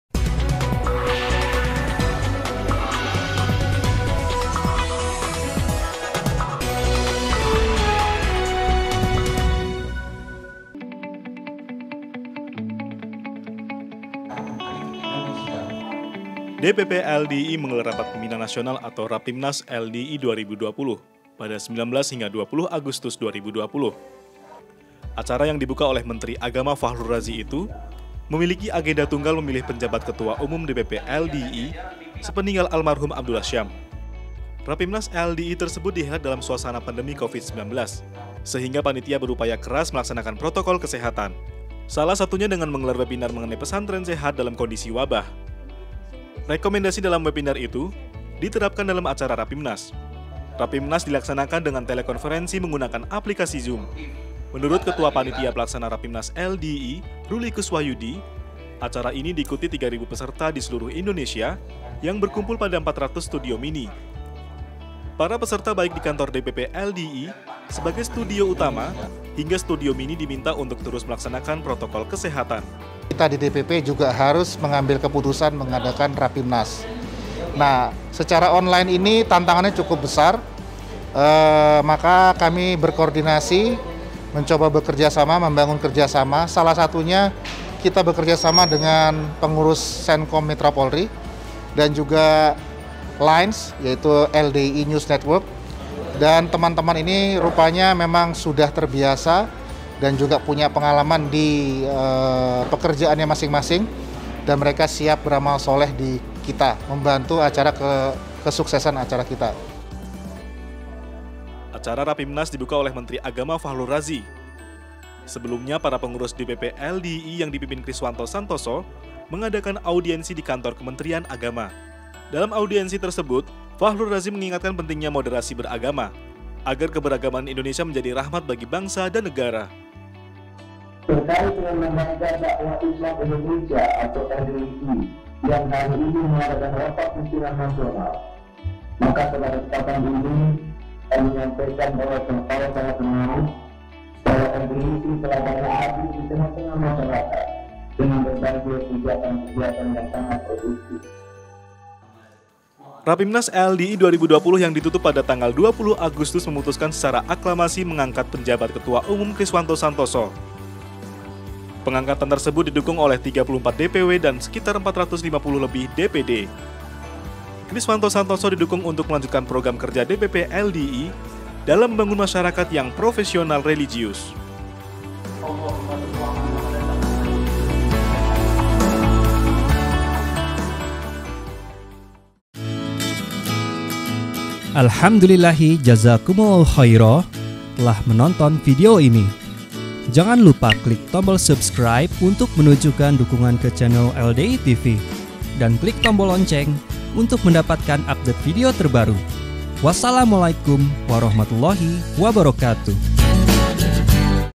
LDII TV: LDII Helat RAPIMNAS, Pilih Pj Ketua Umum
DPP LDII menggelar Rapat Pimpinan Nasional (Rapimnas) LDII 2020, pada 19-20 Agustus 2020, yang digelar secara daring (online) dari studio Kantor DPP LDII, Jakarta. Acara yang mengusung tema ‘Kontribusi Berkelanjutan Untuk Indonesia Bangkit dan Maju’ ini, diikuti sebanyak 3.000 peserta dari seluruh Indonesia.